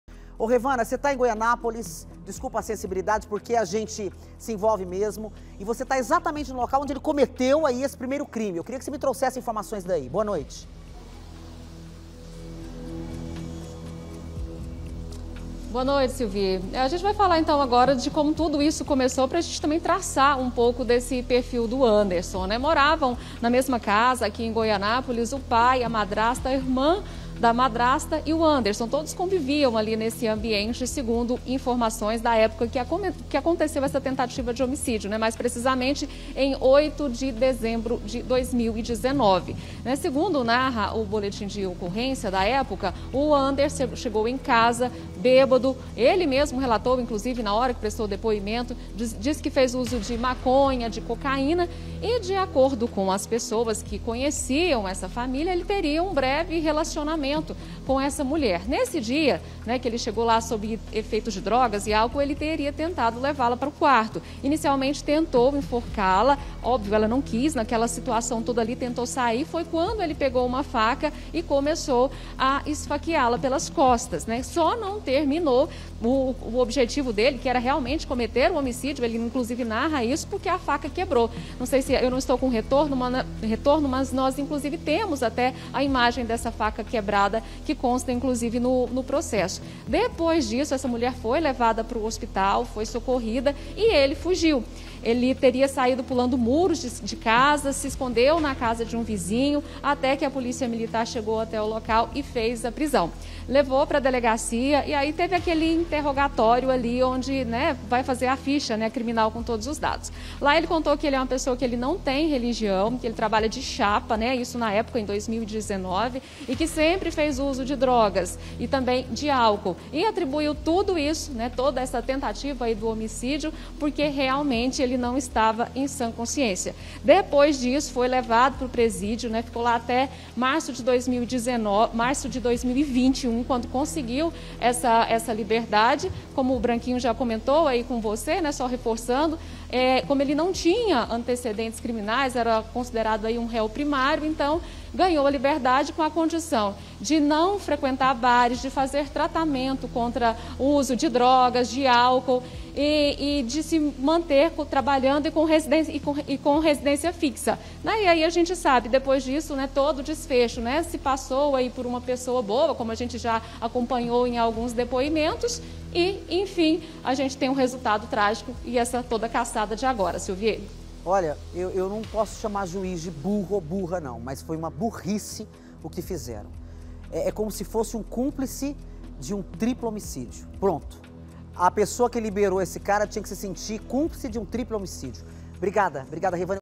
WANDERSON: CASEIRO TENTOU MATAR UMA MULHER EM 2019
EM 2019, WANDERSON FOI PRESO PELO CRIME DE TENTATIVA DE FEMINICÍDIO CONTRA A CUNHADA DO PAI DELE. NA OCASIÃO ELE DEU VÁRIOS GOLPES DE FACA NA VÍTIMA.